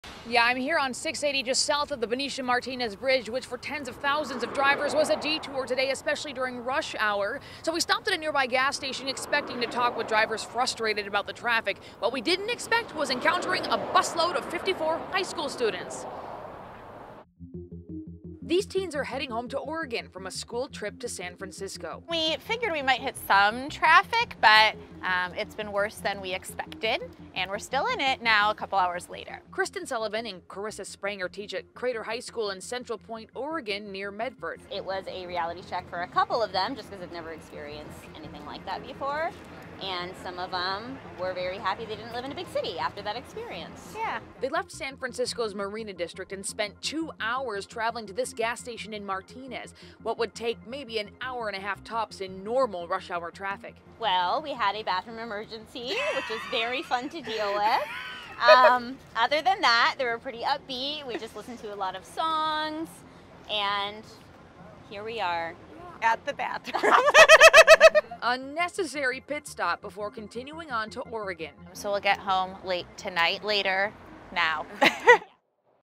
Fire that shutdown I-80 stalls trip home for bus load of Oregon high schoolers
A group of students on the way home from San Francisco's Marina District got caught in a road closure triggered by a fuel tank fire in the East Bay.